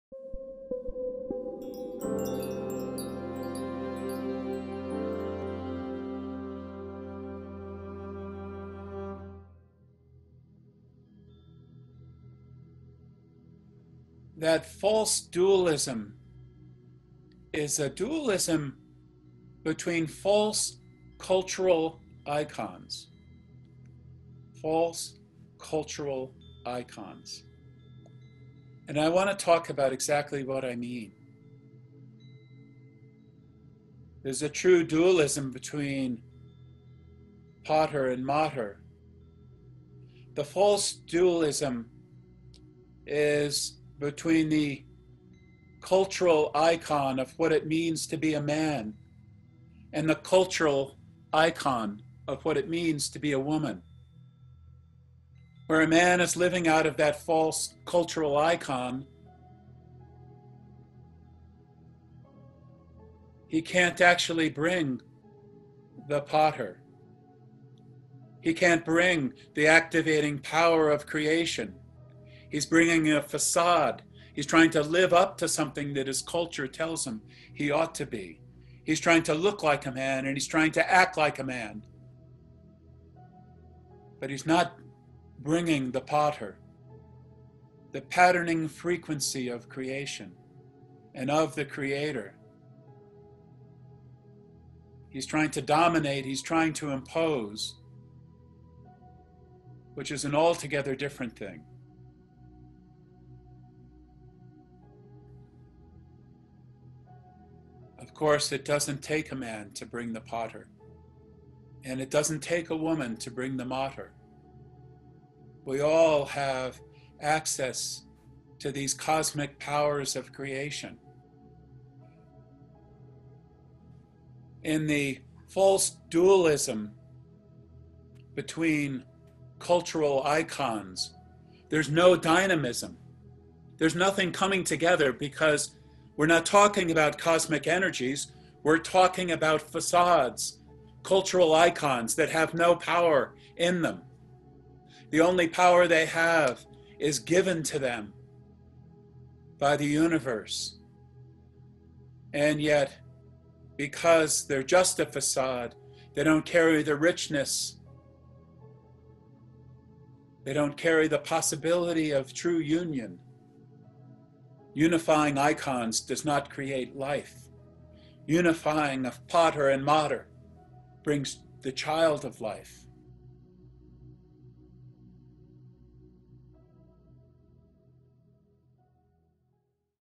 David Karchere: False Dualism Between Cultural Icons
July 5, 2020: Spiritual Director David Karchere speaks about the false dualism between cultural icons in this excerpt from Sunday service at Sunrise Ranch, Loveland, Colorado.
 Part of our Quarantine Series, David was speaking from his home on the Ranch.

SunriseRanch.org